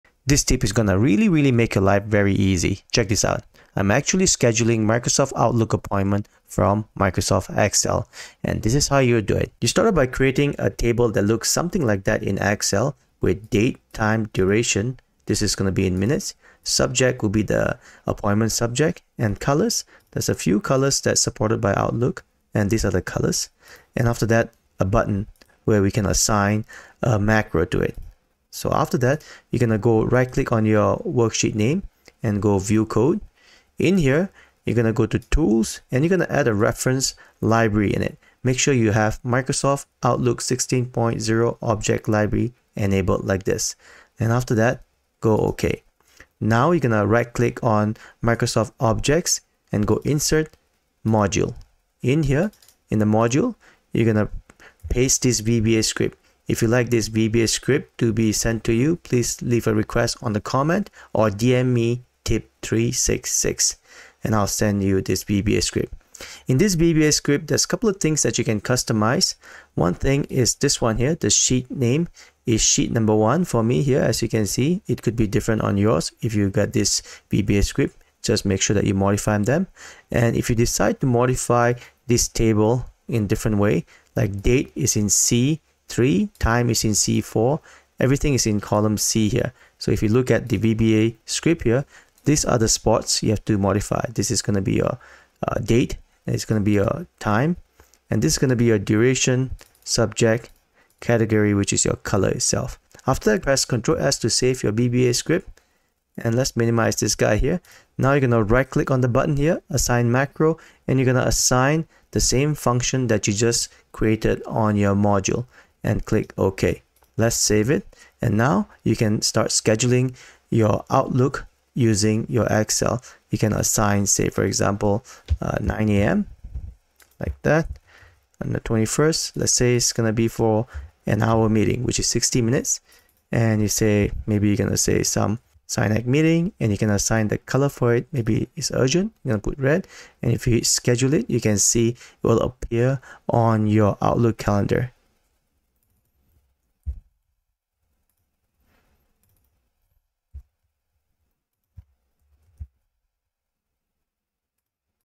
Booking System Using Excel - Excel Tips and Tricks - Detail Explanation
Learn how to create a booking system using Excel.

Creating an appointment schedule in Excel involves several steps. First, you can utilize the spreadsheet's grid layout to represent time slots and days. Enter the relevant information, such as appointment details or names, into the corresponding cells. To establish a booking system in Excel, consider integrating data validation to prevent double bookings and enhance user-friendliness. For a more dynamic scheduling program, utilize Excel's conditional formatting to highlight booked slots or upcoming appointments. Additionally, explore the use of formulas or macros for automated reminders. While Excel does not inherently offer a dedicated scheduling template, you can find and customize pre-existing templates online or design your own based on the specific needs of your scheduling system.

Here are the steps outlined in my video.

Setup MS Outlook Appointment From Excel
1) Right-click sheet ~ View Code
2) Tools ~ References...
3) Microsoft Outlook 16.0. Object Library
4) Ok
5) Right-click on Microsoft Excel Objects
6) Insert ~ Module
7) Paste VBA
8) Ctrl + S
9) Close VBA editor
10) Assign macro to button